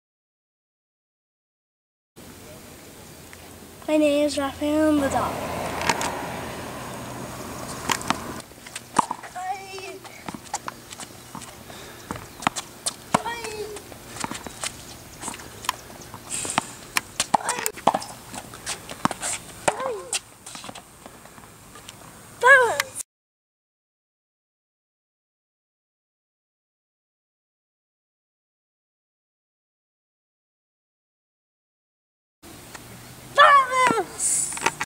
Rafael Nadal, by Jared, age 6
Jared impersonates his favorite tennis player